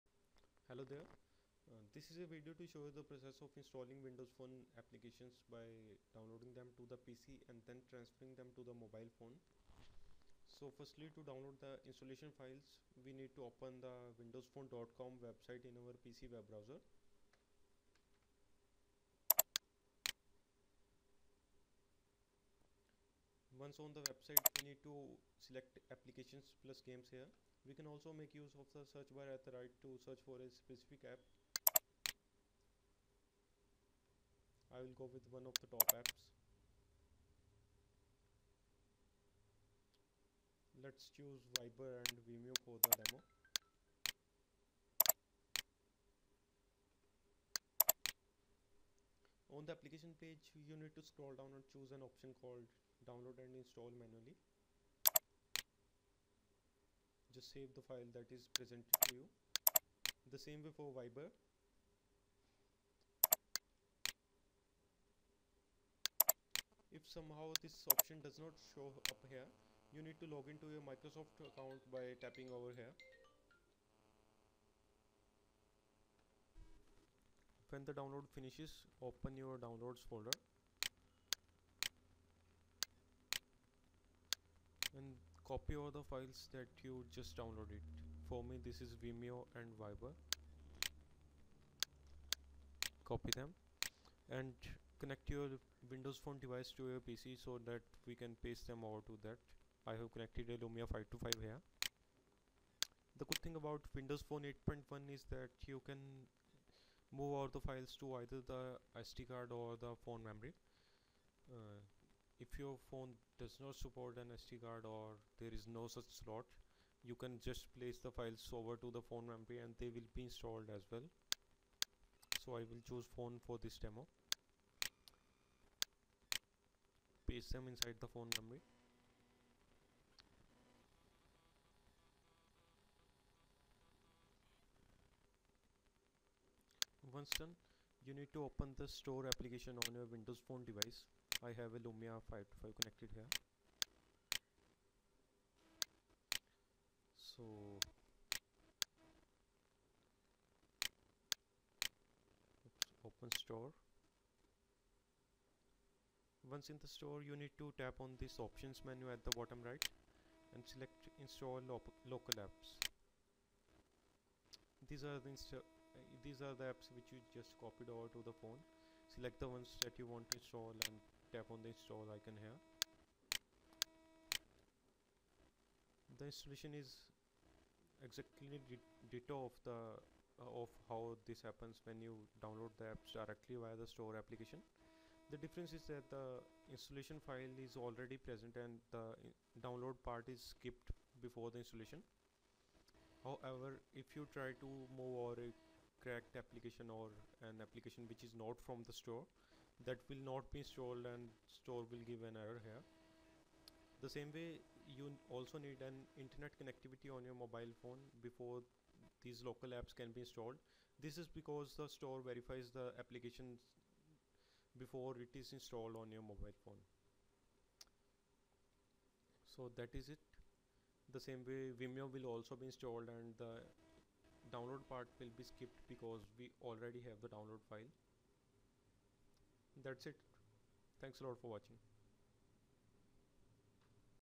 Download and Install Apps (XAP/APPX Files) on WP8.1 via PC
Quick walkthrough of the process of iunstalling the Windows Phone apps by downloading them to the PC and then sideloading the XAP or APPX Files on the Windows Phone.
Windows Phone 8.1 has hugely improved this feature and copied installation files appear up almost instantly in the Store, unlike the way it happened earlier with Windows Phone 8.0 and required resatrt.